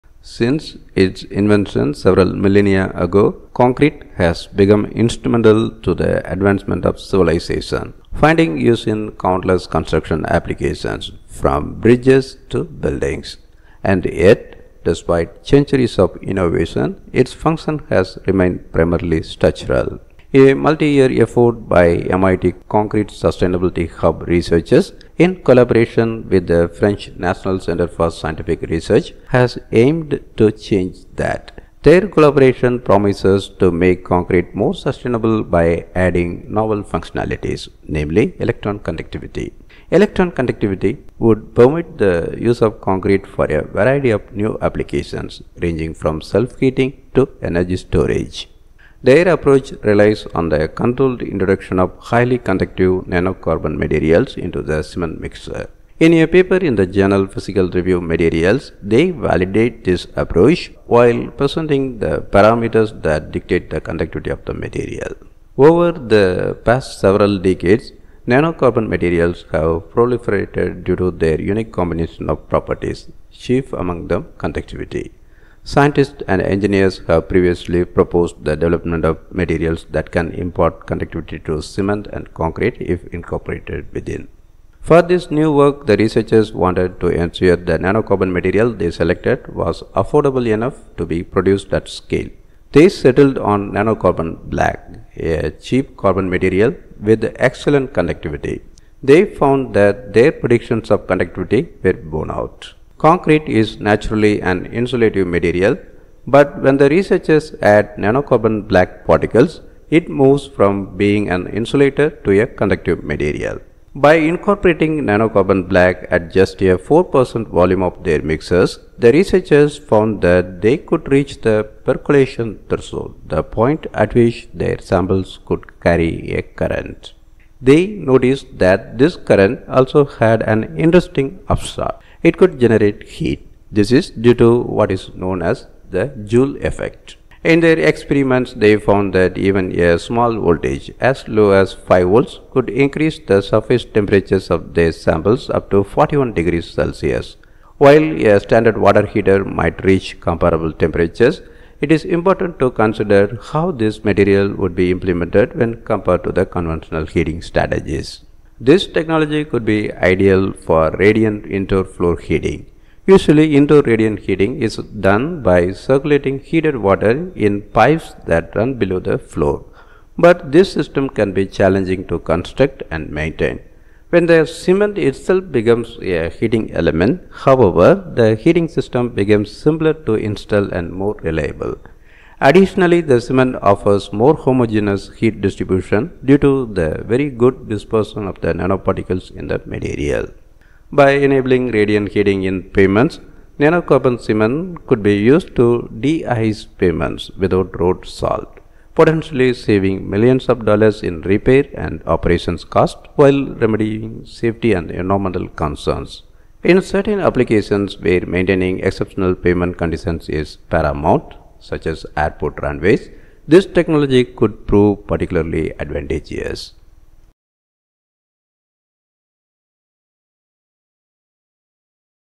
Electrifying Cement with Nanocarbon Black | Conductive Cement
A research collaboration of MIT and CNRS promises to make concrete more sustainable by adding novel functionalities — namely, electron conductivity. Electron conductivity would permit the use of concrete for a variety of new applications, ranging from self-heating to energy storage

Credit: Andrew Logan

Research Paper: Electric energy dissipation and electric tortuosity in electron conductive cement-based materials